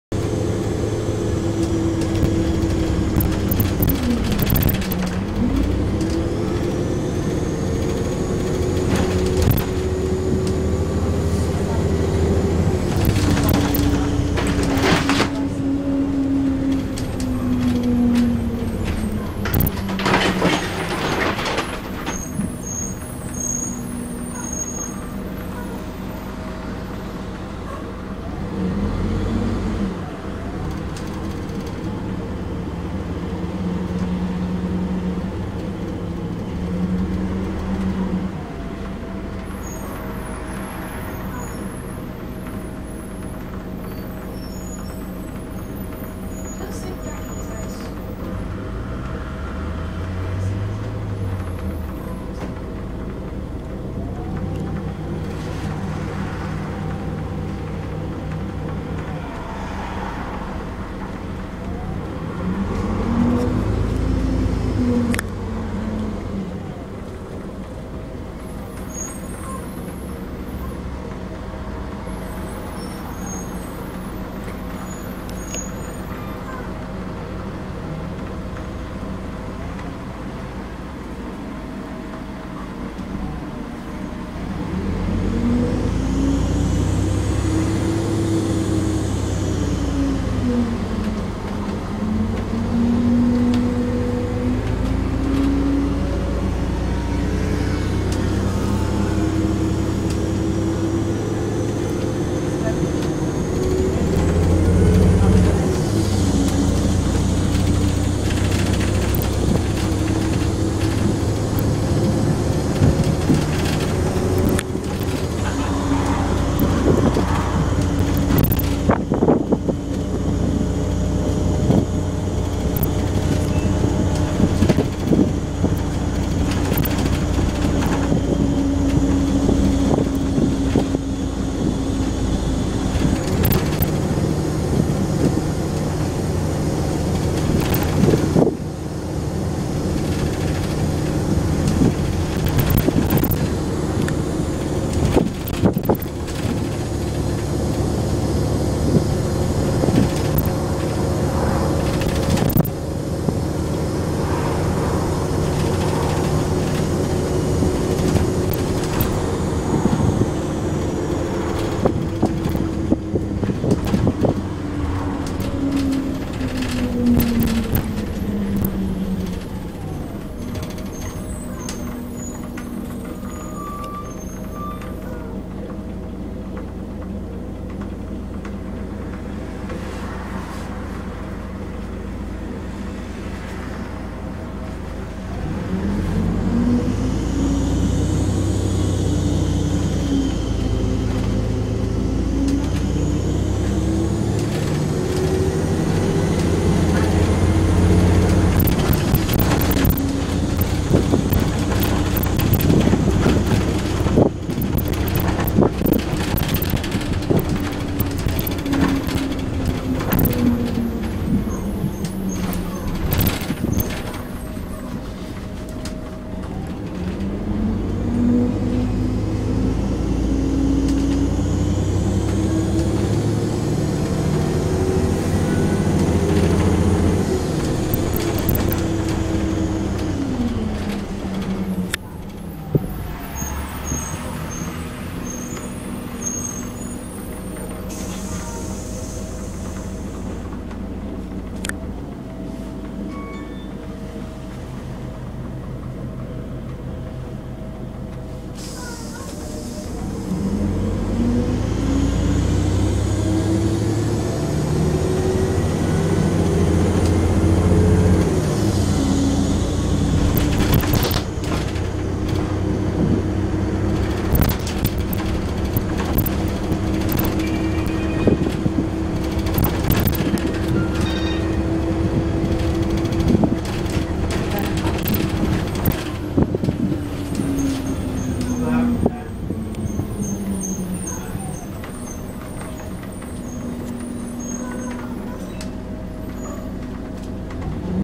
Stagecoach Scunthorpe 16465 S165RET
Onboard this Volvo Olympian Bus taken on service 32 from Lodge Moor to Bus station the Bus whistles a bit on